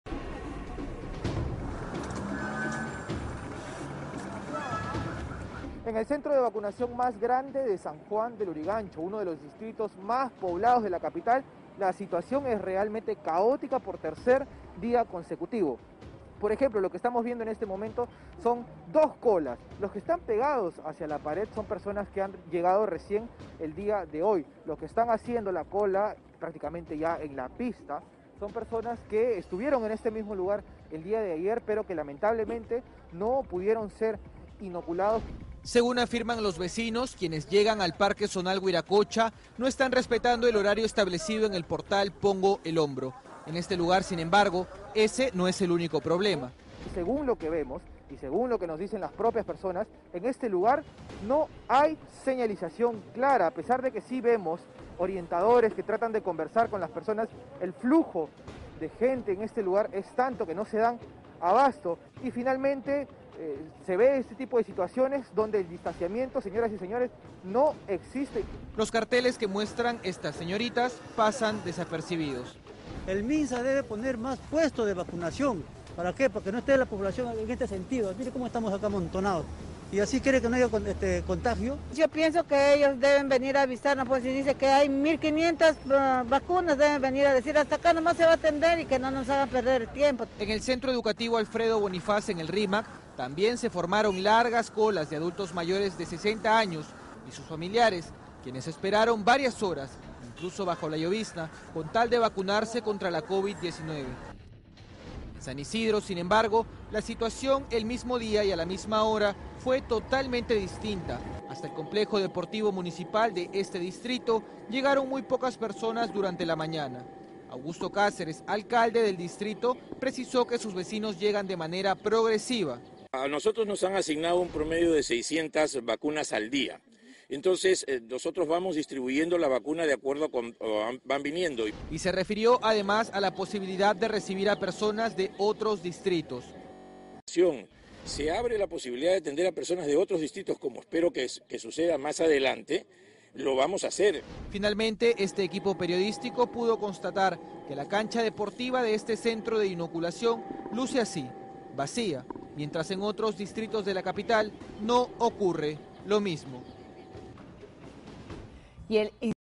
Largas colas y caos se pudo apreciar en diversos vacunatorios de Lima
Largas colas y caos es lo que un equipo de Panamericana Televisión pudo constatar en diversos vacunatorios de Lima Metropolitana.

Por ejemplo, en el Parque Zonal Huiracocha, ubicado en San Juan de Lurigancho (SJL), habían dos enormes filas, una conformada por personas que les tocaba ser inoculadas este miércoles y otra donde estaban los ciudadanos que no pudieron ser vacunados el día anterior. Sin embargo, no fue el único problema, pues algunos de los presentes se quejaron porque no había una clara señalización.

Mientras tanto, en el centro educativo Alfredo Bonifaz, en el distrito de Rímac, el panorama que se vivía era similar.